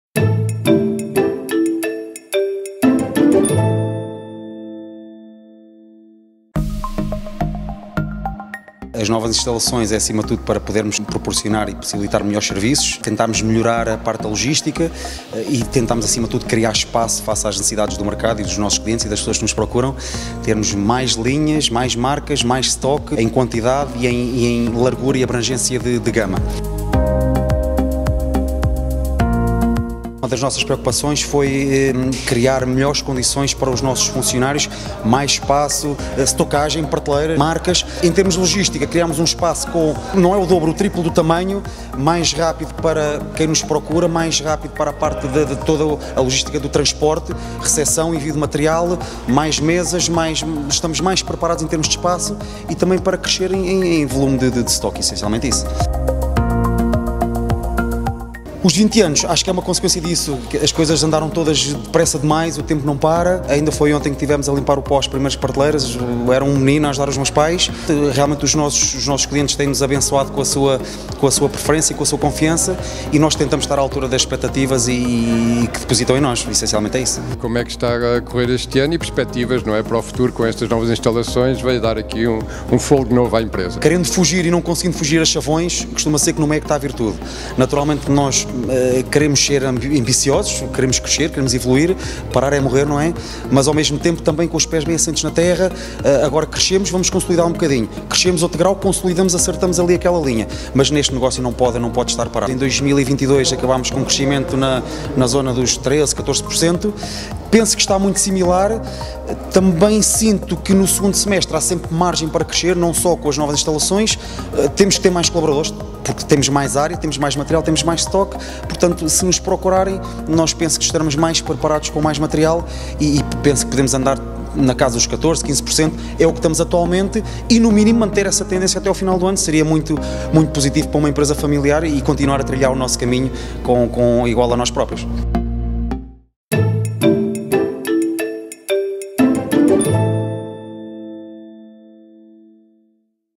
Filourém apresenta novas instalações
Para Carlos Gonçalves, gerente da Filourém “a remodelação das instalações são uma consequência da evolução da empresa, que nos últimos anos tem tido crescimentos de dois dígitos.